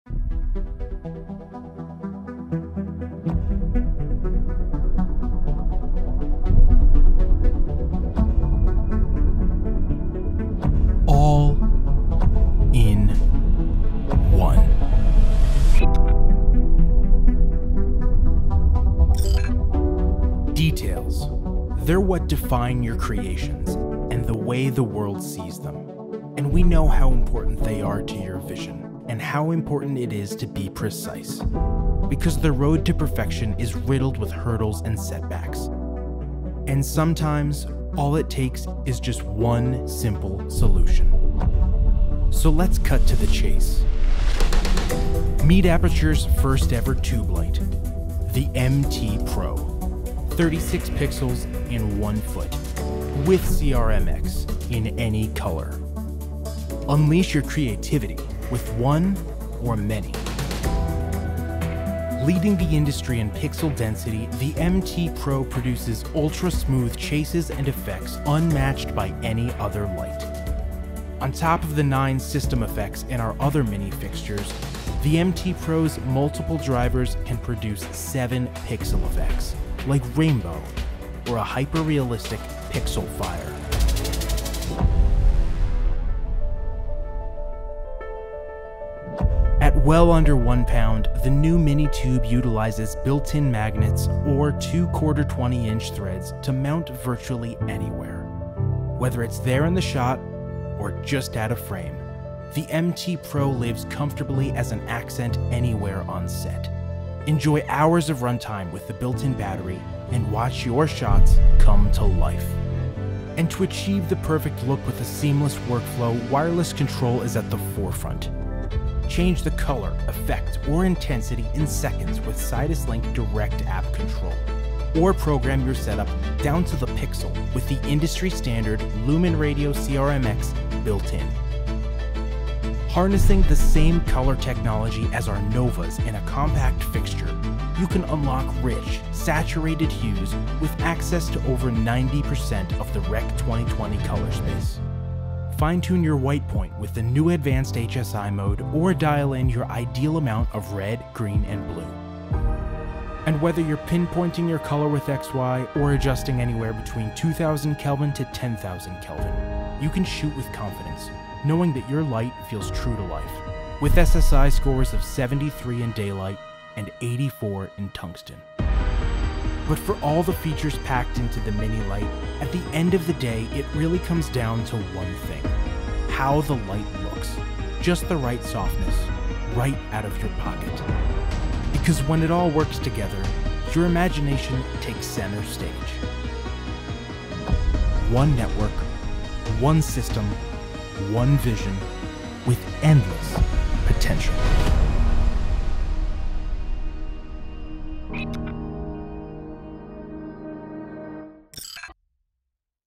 Introducing the MT Pro | Pixel Perfect
The MT Pro is Aputure’s first-ever full-color mini LED tube light. Condensed into its compact 1-foot frame, the MT Pro shines brightly with professional features that stand above the rest –including industry-leading pixel density and wireless CRMX connectivity. The MT Pro is the perfect fit for all levels of filmmakers, photographers, and content creators –use it as an in-camera practical, on-camera fill light, hidden accent, or make it the center of attention for on-the-go content creation and product cinematography. Whether for its compact size or abundance of professional features, the MT Pro has a place in every creative’s toolkit, from lighting technicians to creators.